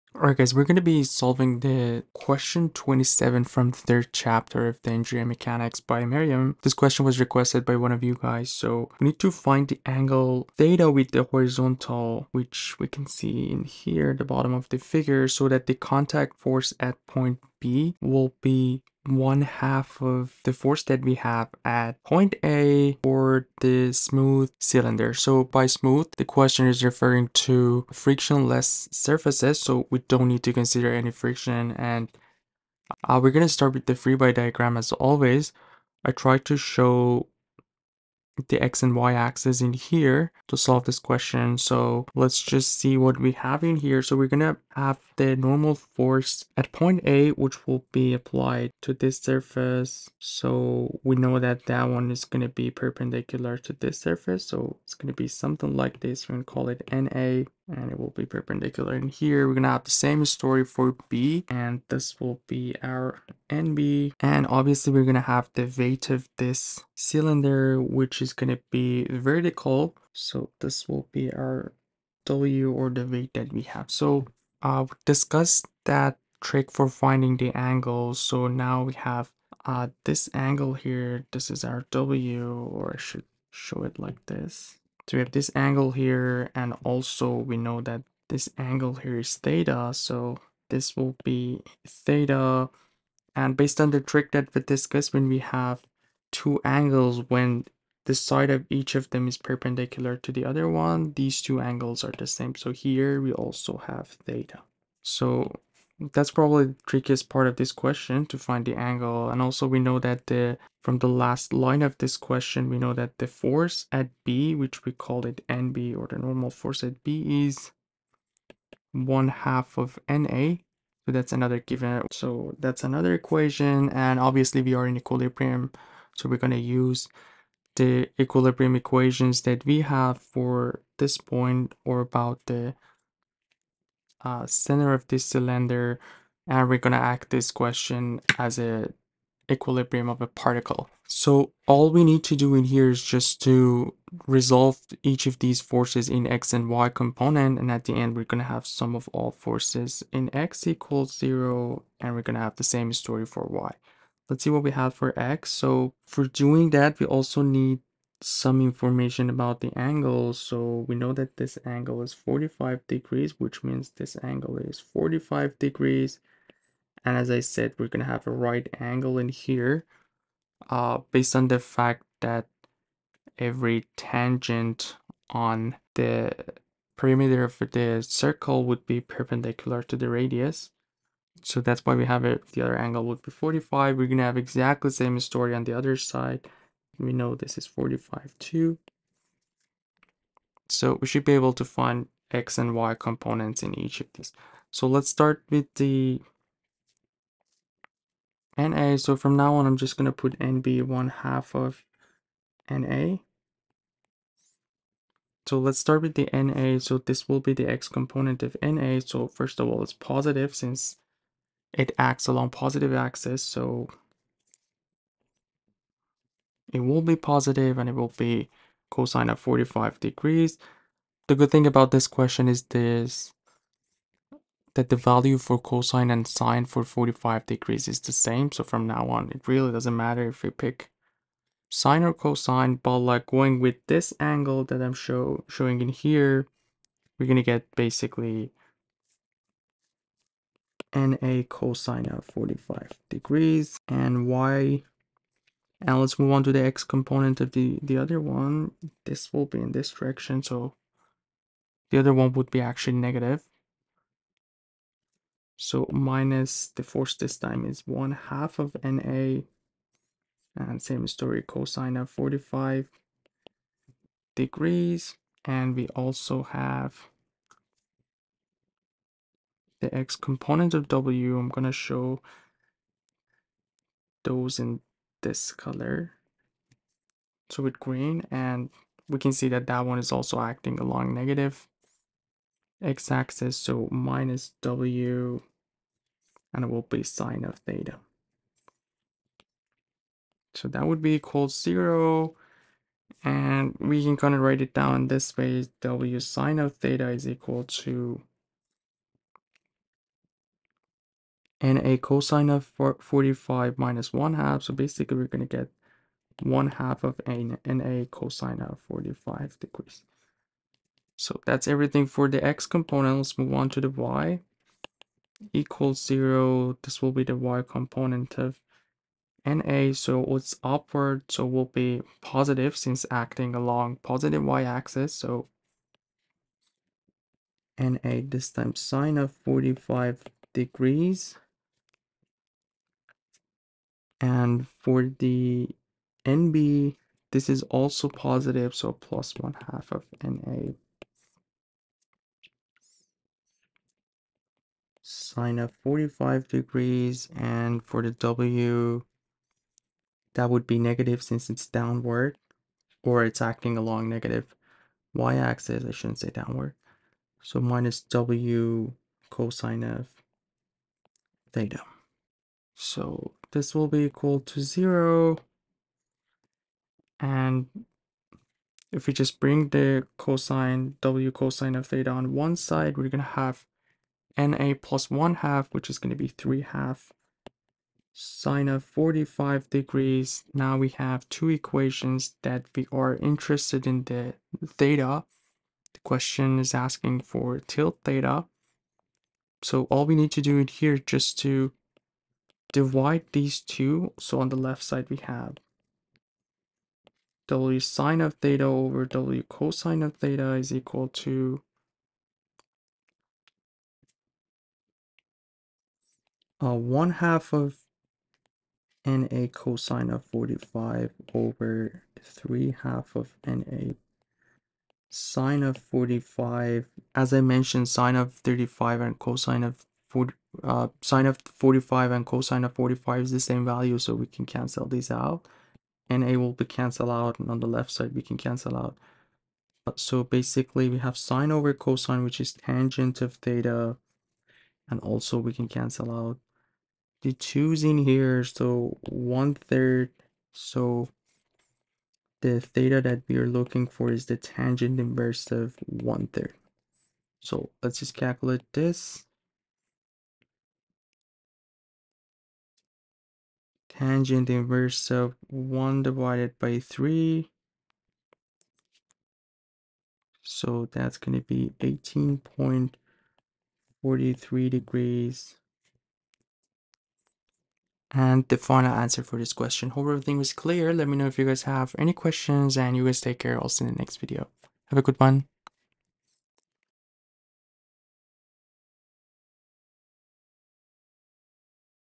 Solved Problem 3.27 | Find the angle of tilt 𝜃 with the horizontal so that the contact force at B
Solved Problem 3.27 | Engineering Mechanics-Statics-8th edition-J.L. Meriam & L.G. Kraige:

Find the angle of tilt 𝜃 with the horizontal so that the contact force at B will be one-half that at A for the smooth cylinder.

💡Textbooks Covered In This Channel💡